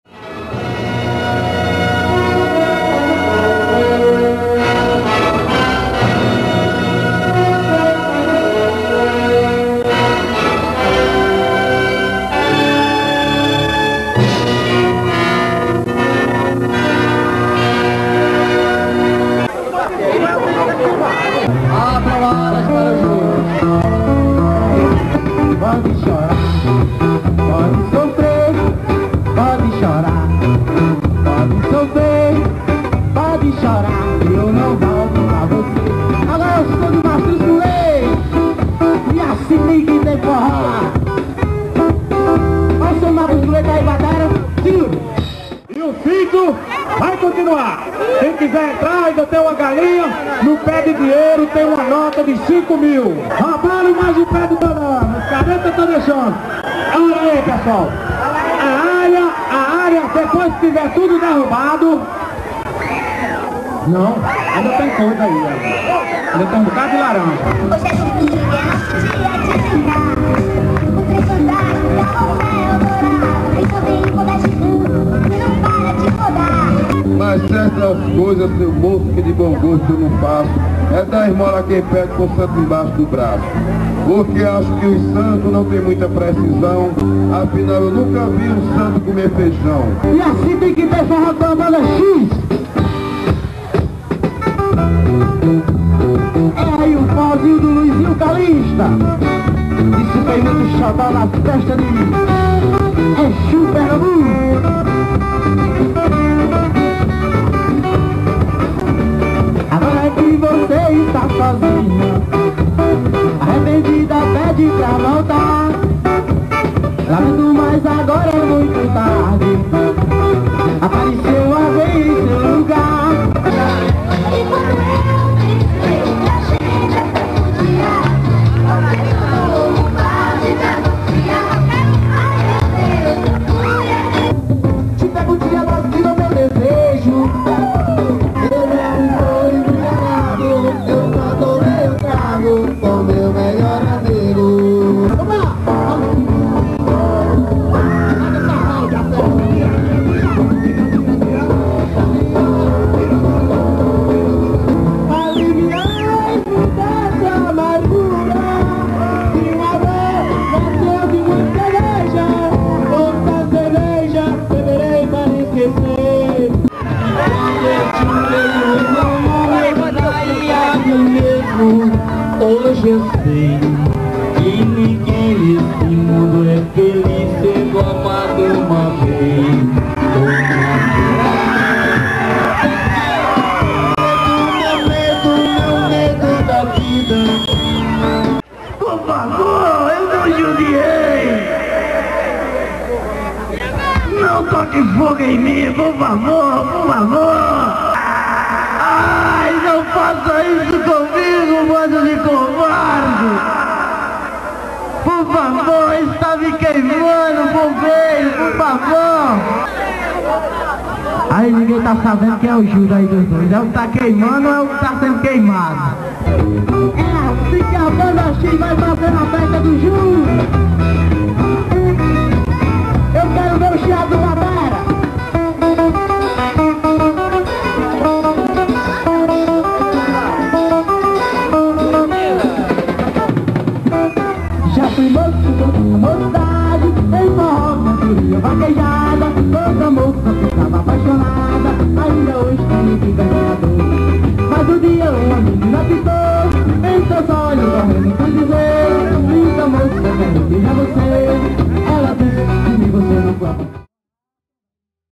JUDAS MALHAÇÃO 1992 - Juazeiro do Norte/Ceará
Grupo Resgatar, sediado no bairro Socorro na cidade de Juazeiro do Norte, Ceará, faz a malhação do Judas há mais de 30 anos. Criou o famoso "çítiu do Judas". A TV padre Cicero transmite todo ano essa manifestação cultural do interior do Nordeste.